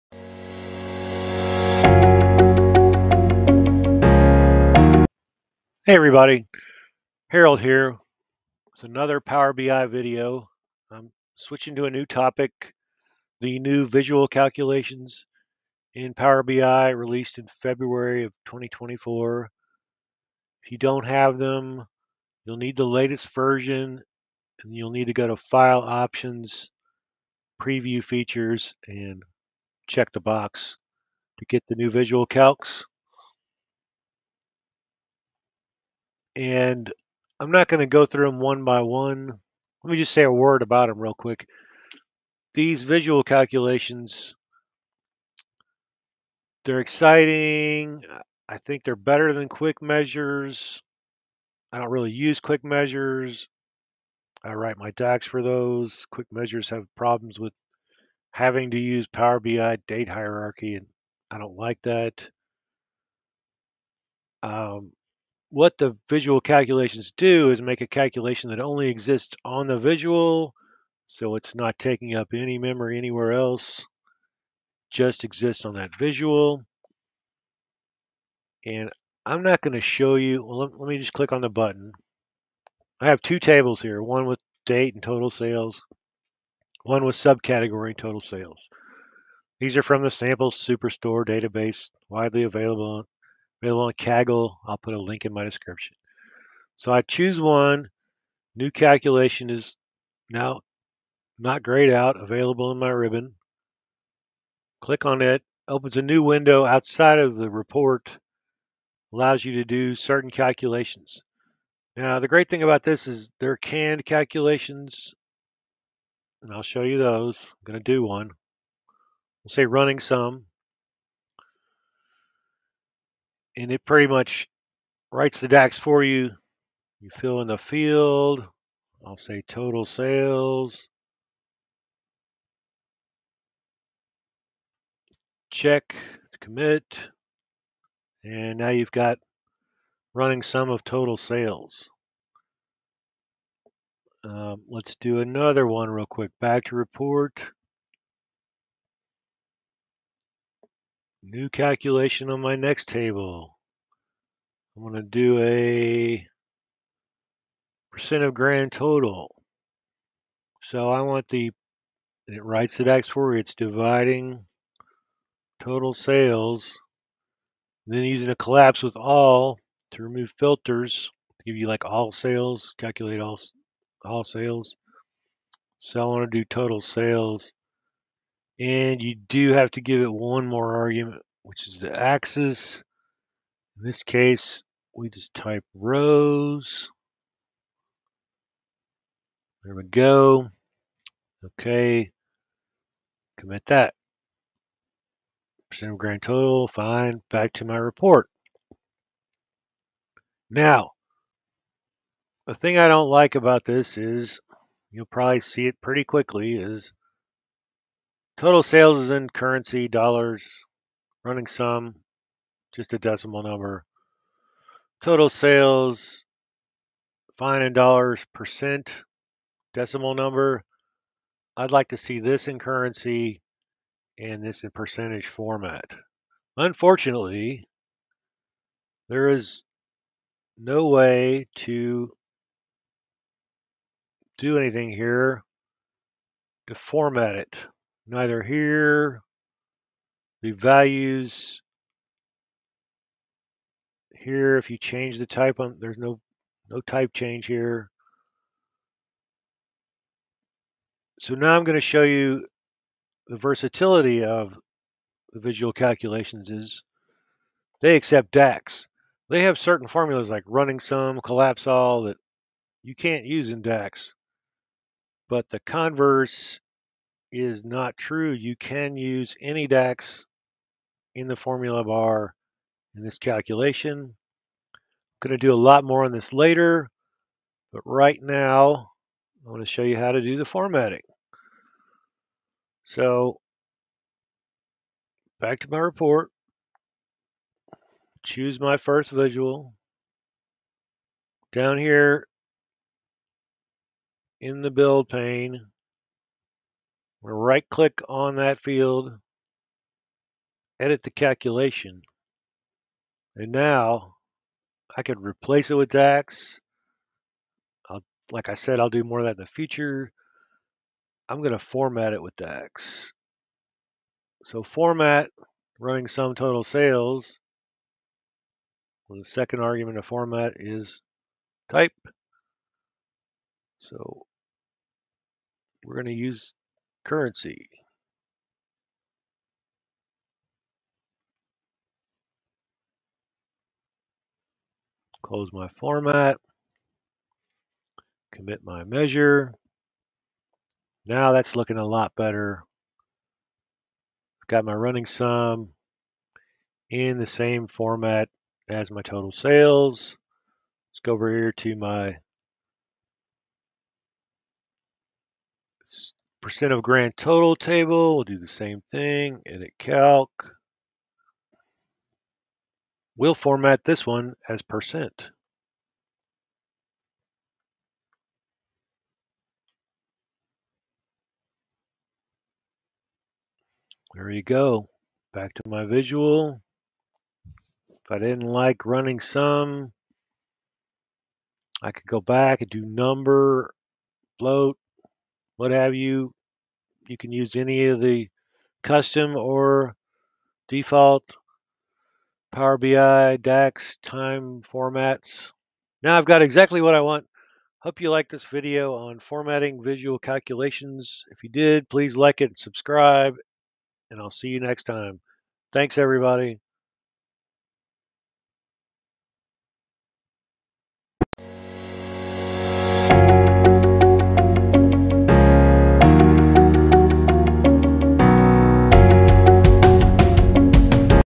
Power BI Visual Calculations: Number Formatting
How to format number values in the new Power BI Visual Calculations.  Currency, Percent, etc.   You need to use DAX.

Sample Superstore dataset can be found at Kaggle.com.